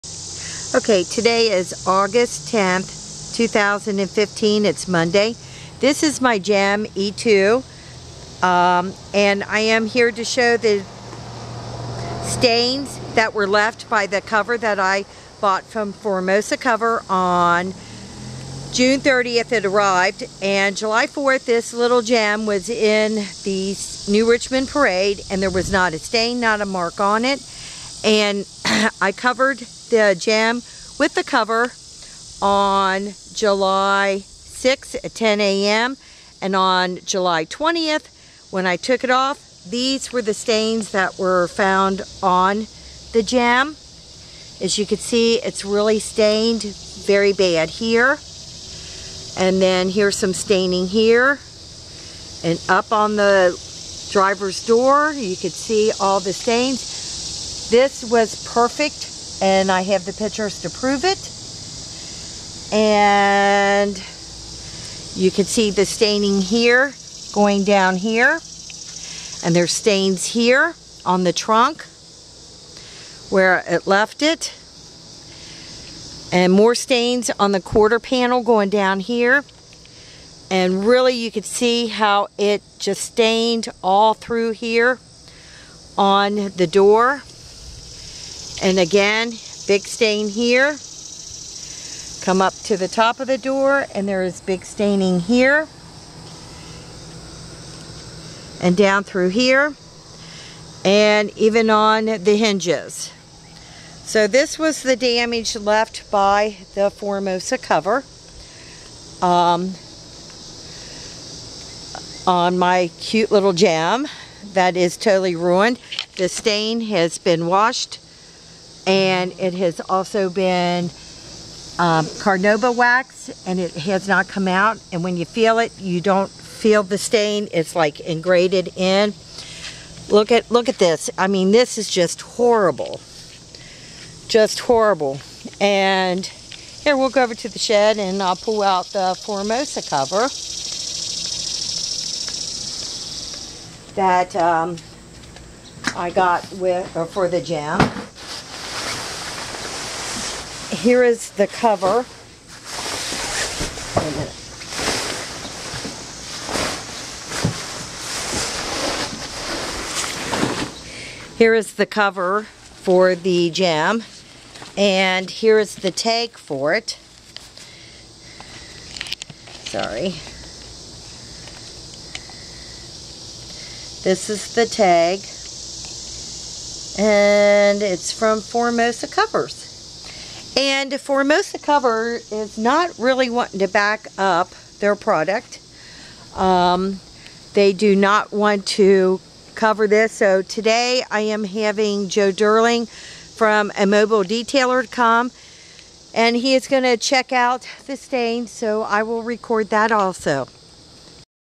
Damage cause to my Gem e2 done by a FORMOSA COVER.
This video shows the damage that was done to my Gem e2 by a cover made by FORMOSA COVER and purchased thru Amazon.com.
The cover that I purchased was black  and I placed the cover on my Gem e2 on Monday July 6 2015 and we had rain many times from that day until Monday July 20 2015 when I took it off.  I was totally upset and shocked when I saw the condition of my Gem e2. All of the white areas on my Gem was covered with stains from where the dye from the cover seeped through.    My lovely GEM e2 was ruined by the cover that was purchased to protect it!  As of yet FORMOSA COVER has not done anything to fix the problem.